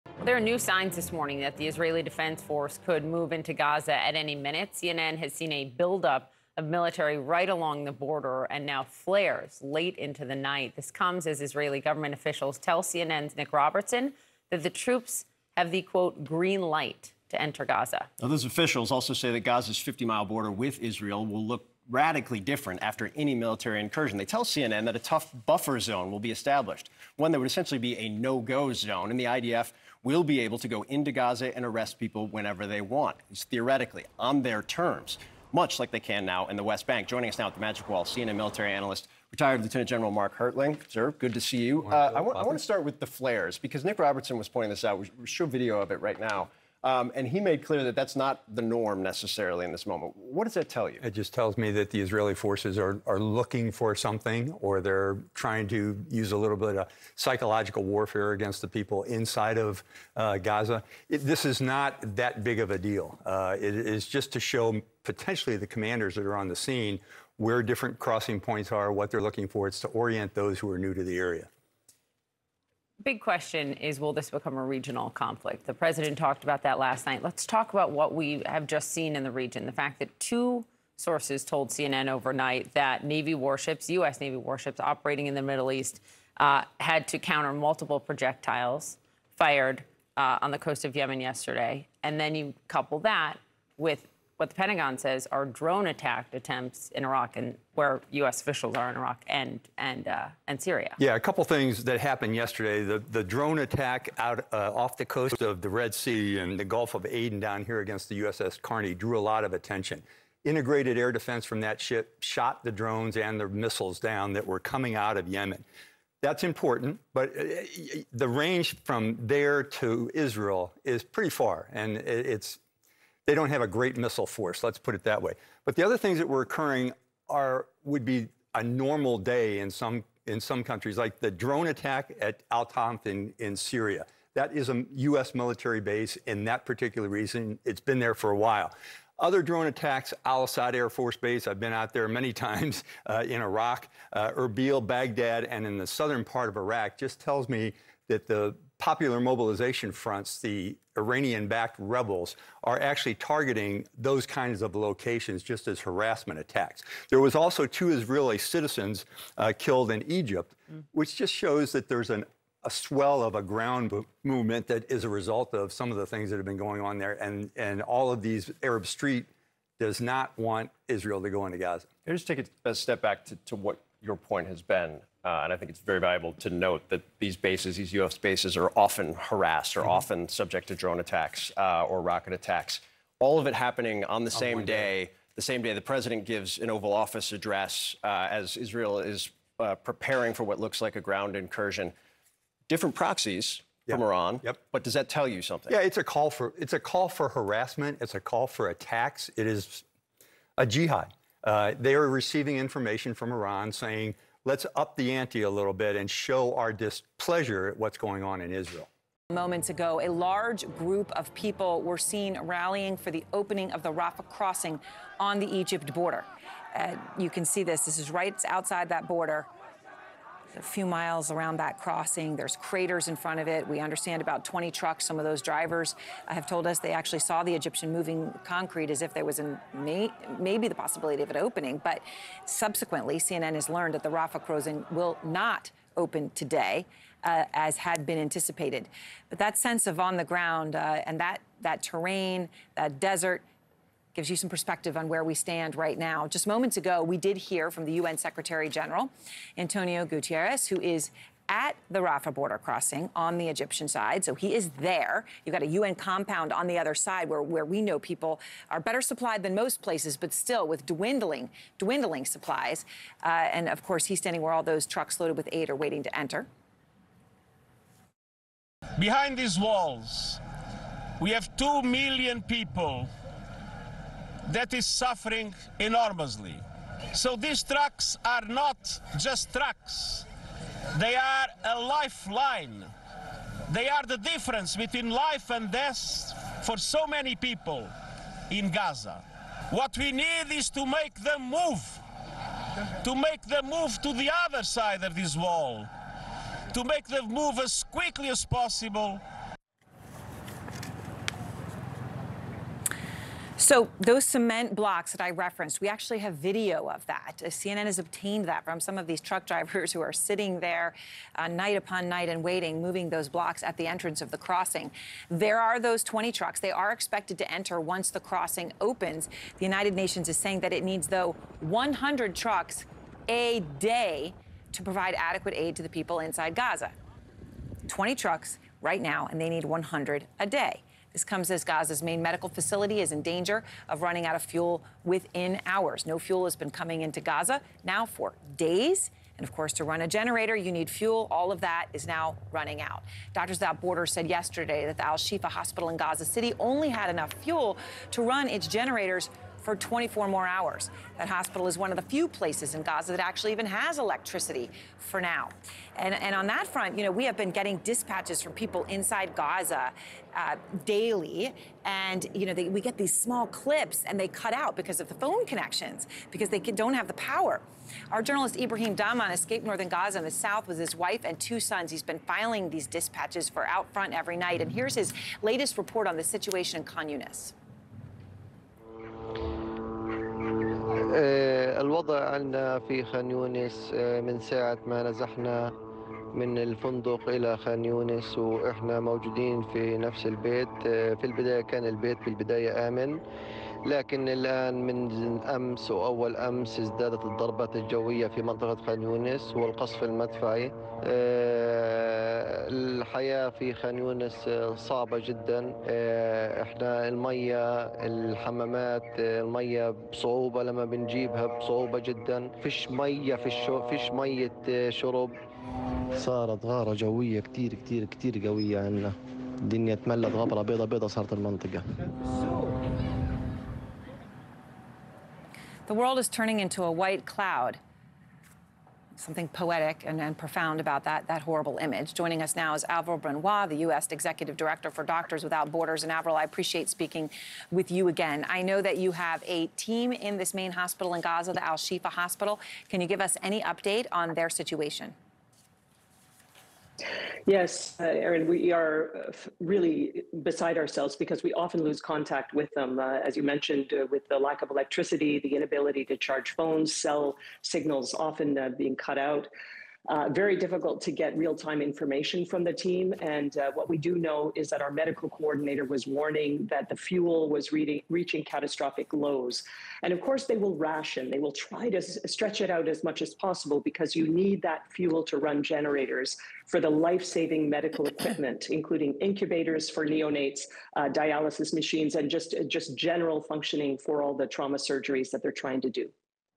Attacks on US military in Middle East stoke fear of regional war
The Pentagon press secretary Air Force Brig. Gen. Pat Ryder confirmed the USS Carney shot down three land attack missiles as well as several drones that were launched by Iran-backed Houthi forces in Yemen. The incident was one of a series in recent days with US bases being targeted by drones in Syria and Iraq amid increasing tensions in the region as the war between Israel and Hamas continues.